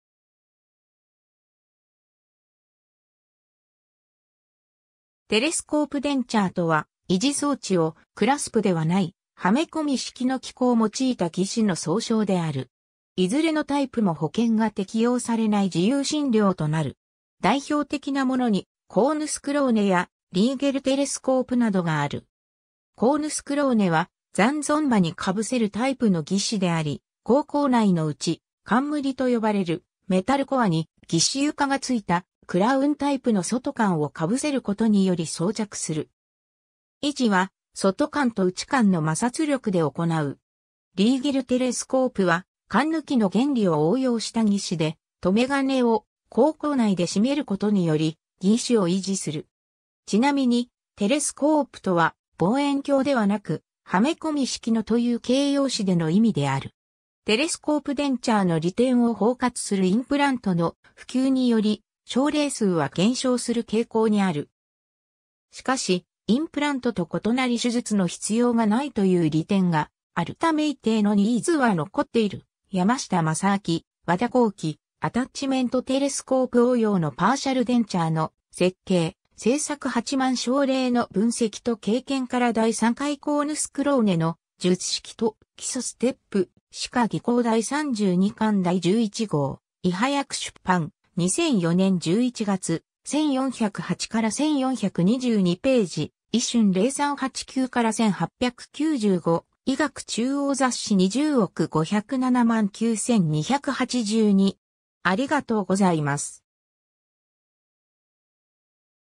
テレスコープデンチャー
テレスコープデンチャーとは、維持装置をクラスプ（バネ）ではない、はめ込み式の機構を用いた義歯の総称である。
いずれのタイプも保険が適用されない自由診療となる。
 代表的なものに、コーヌスクローネやリーゲルテレスコープなどがある。
コーヌスクローネは残存歯に被せるタイプの義歯であり、口腔内の内冠と呼ばれるメタルコアに義歯床がついたクラウンタイプの外冠をかぶせる事により装着する。
維持は外冠と内冠の摩擦力で行う。
リーゲルテレスコープは閂の原理を応用した義歯で、留め金を口腔内でしめる事により義歯を維持する。
ちなみにテレスコープとは「望遠鏡」（名詞）ではなく、「はめ込み式の」という形容詞での意味である。
 テレスコープデンチャーの利点を包括するインプラントの普及により、症例数は減少する傾向にある。
しかし、インプラントと異なり手術の必要がないという利点があるため一定のニーズは残っている ^ 山下正晃、和田弘毅「アタッチメント・テレスコープ応用のパーシャルデンチャーの設計・製作　8万症例の分析と経験から　第3回 コーヌスクローネの術式と基礎ステップ」『歯科技工』第32巻第11号、医歯薬出版、2004年11月、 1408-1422頁、 ISSN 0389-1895、 医学中央雑誌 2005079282。